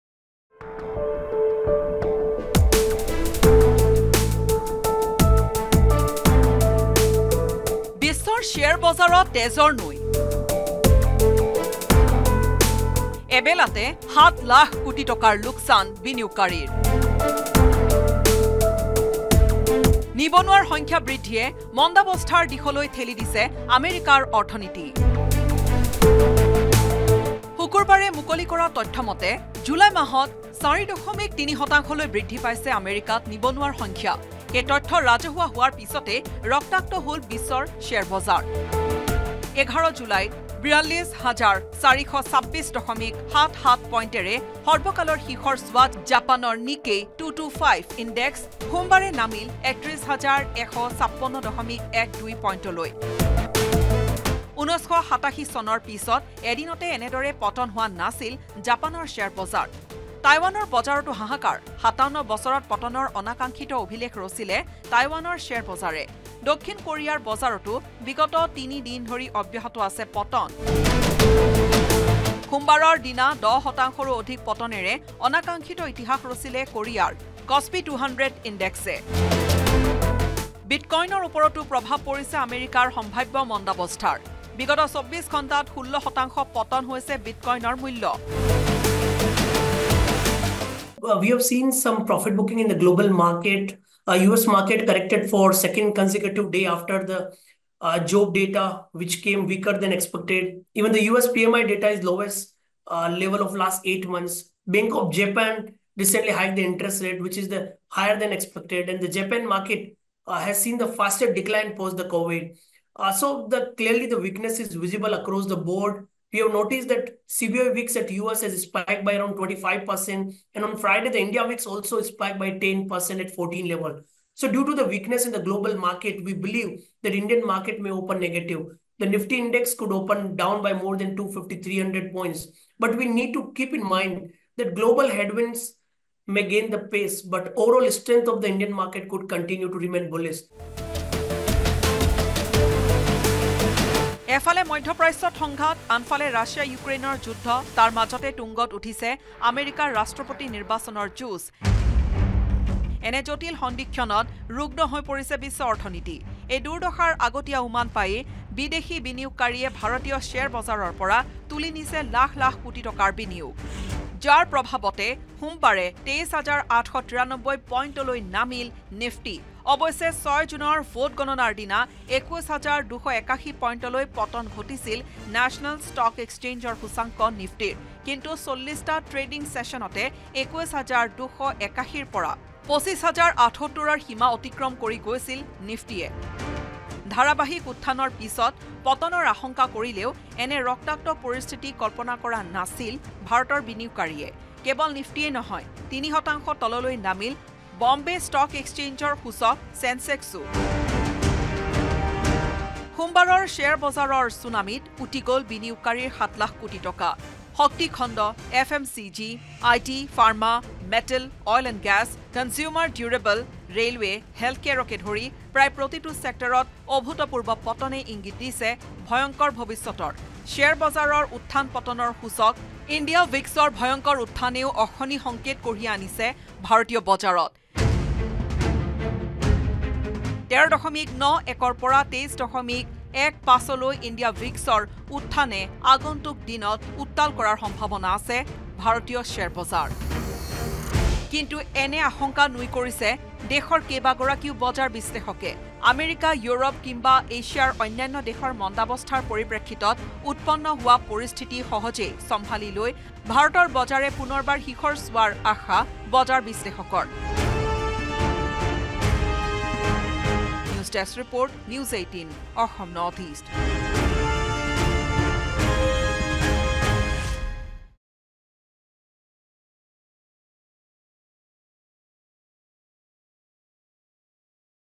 Stock market crash | বিশ্বৰ শ্বেয়াৰ বজাৰত তেজৰ নৈ। N18G
Stock market crash | হঠাৎ নিম্নগামী বিশ্বৰ শ্বেয়াৰ বজাৰ। এবেলাতে ৭লাখ কোটি টকাৰ লোকচান হ’ল বিনিয়োগকাৰীৰ। নিবনুৱাৰ সংখ্যা বৃদ্ধিয়ে মন্দাবস্থাৰ দিশলৈ ঠেলি দিছে আমেৰিকাৰ অৰ্থনীতি।